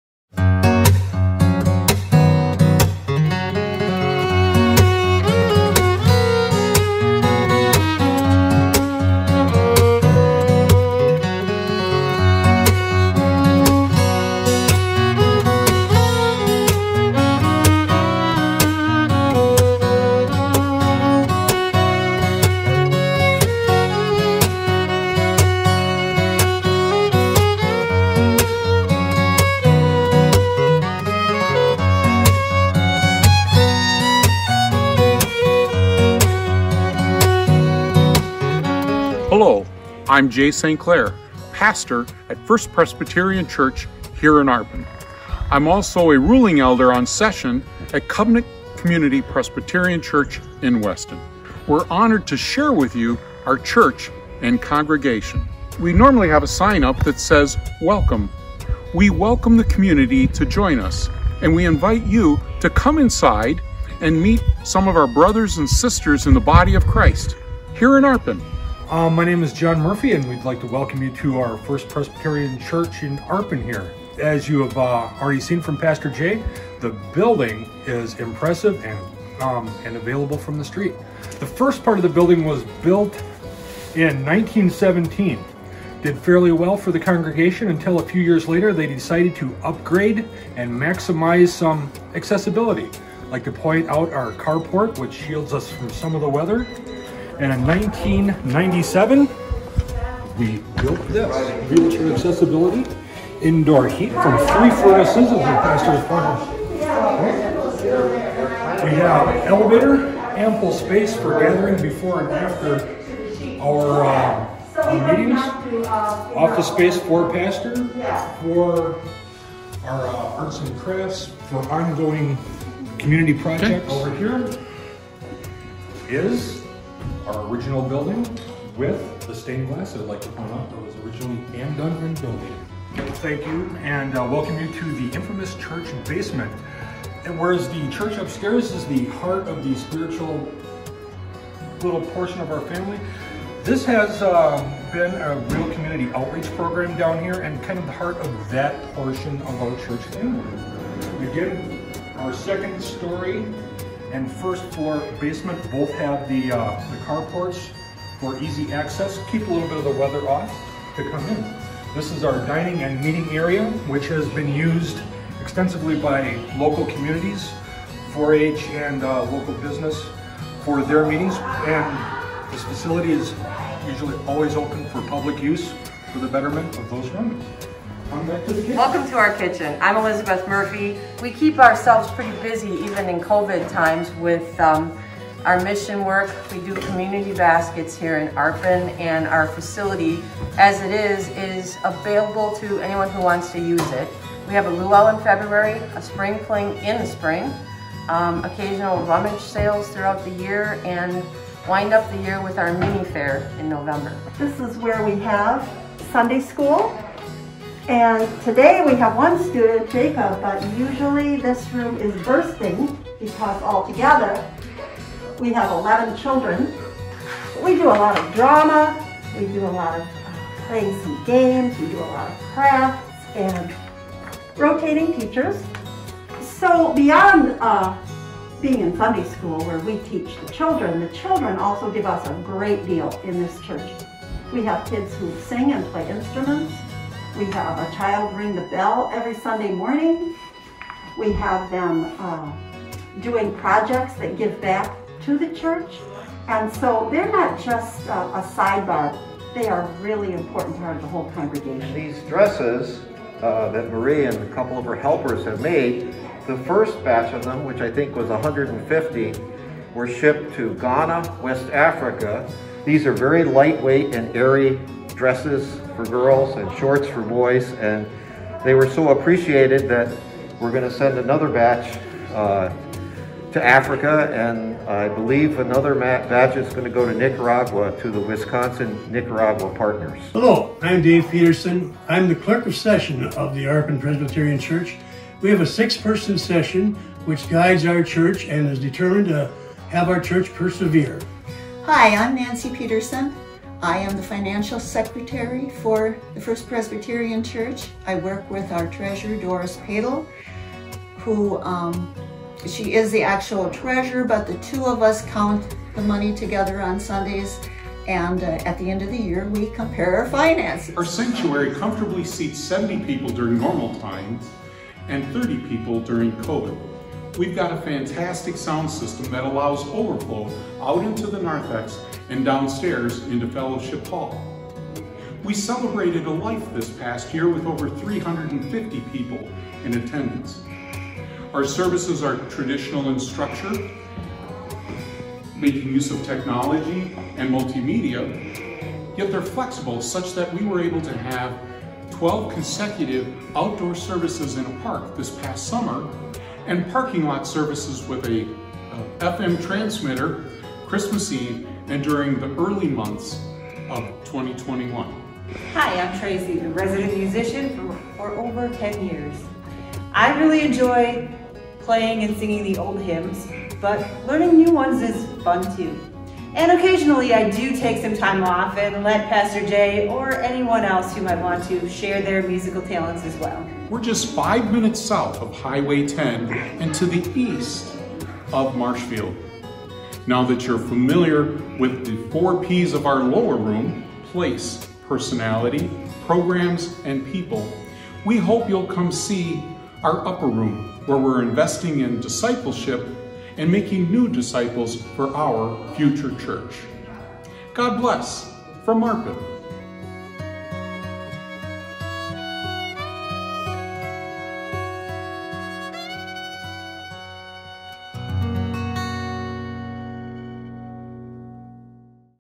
FPC of Arpin
Along with being a Ruling Elder here at CCPC, our very own Jay St. Clair is the Pastor at First Presbyterian Church of Arpin. We are so proud of Jay and the work he is doing shepherding these wonderful brothers and sisters. Arpin is a rural community near Marshfield, Wisconsin